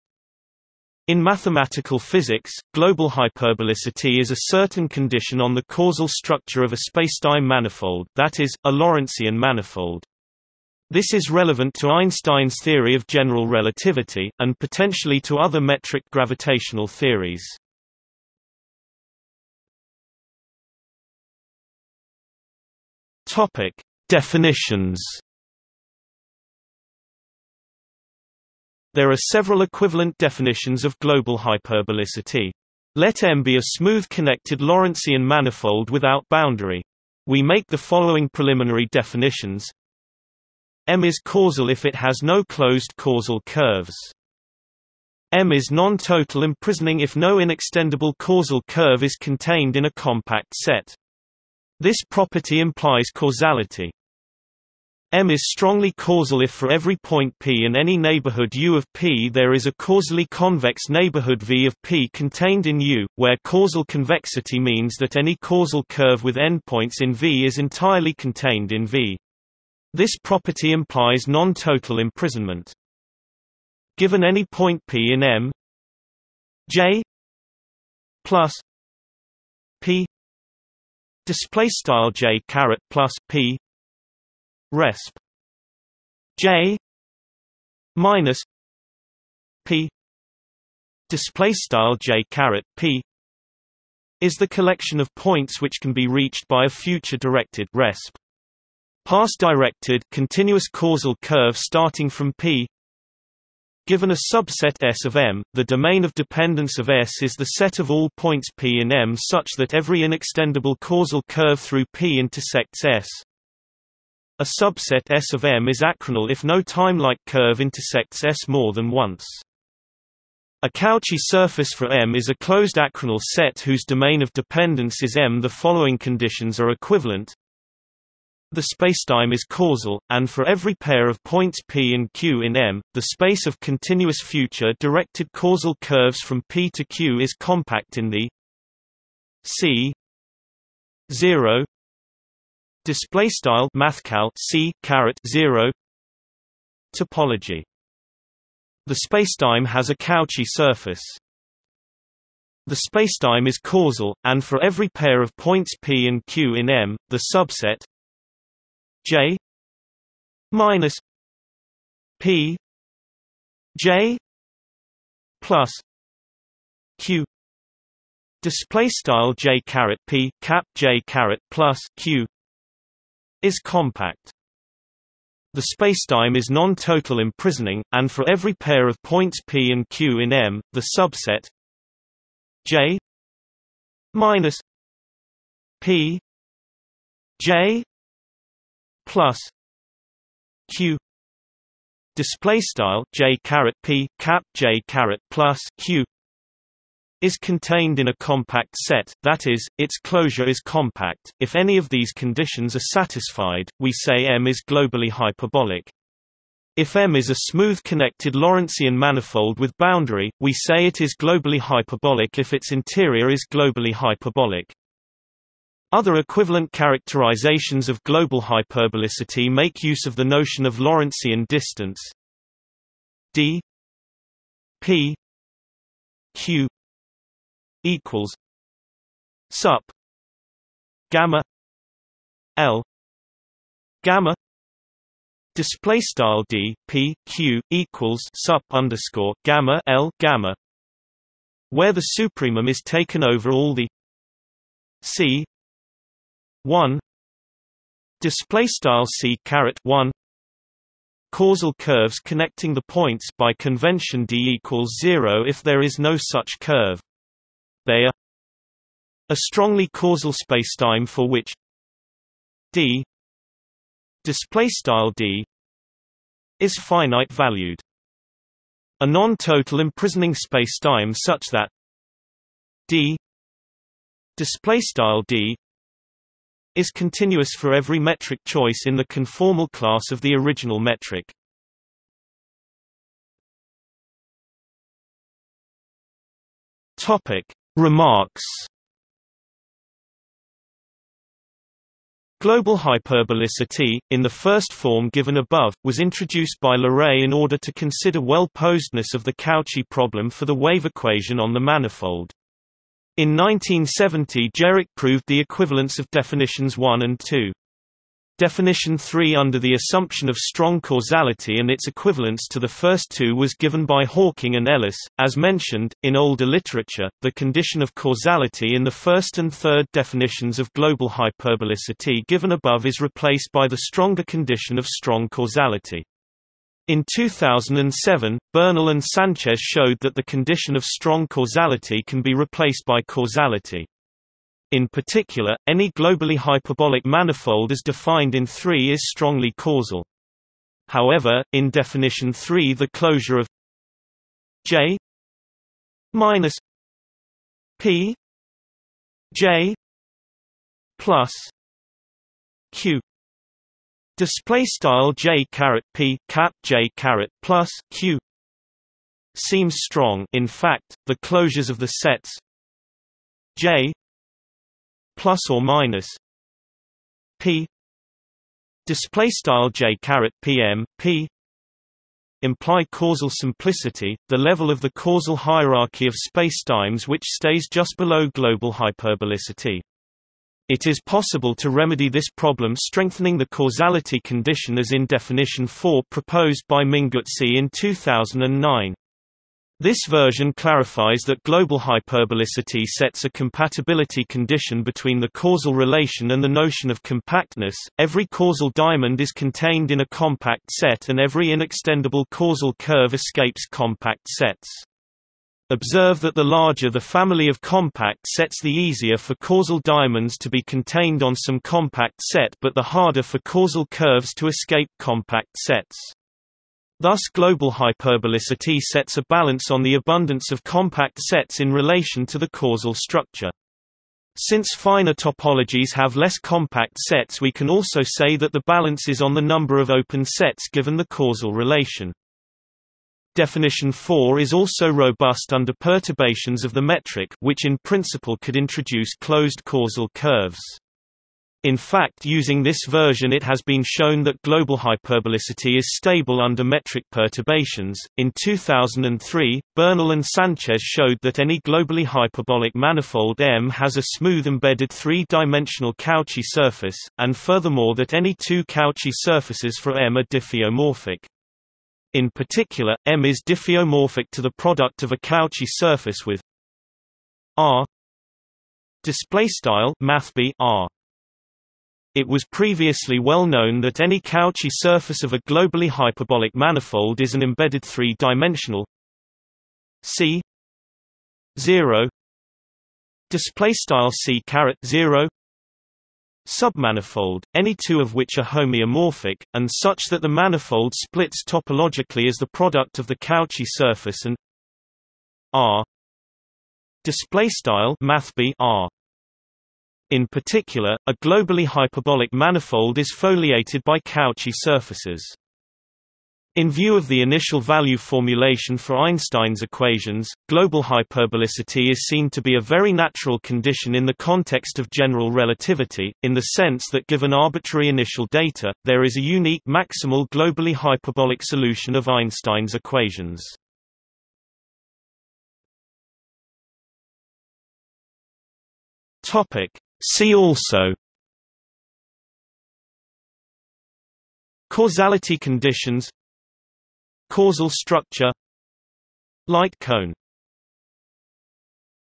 Global hyperbolicity | Wikipedia audio article
00:00:19 1 Definitions
00:04:43 2 Remarks
00:08:45 3 See also

- Socrates

SUMMARY
In mathematical physics, global hyperbolicity is a certain condition on the causal structure of a spacetime manifold (that is, a Lorentzian manifold).  This is relevant to Einstein's theory of general relativity, and potentially to other metric gravitational theories.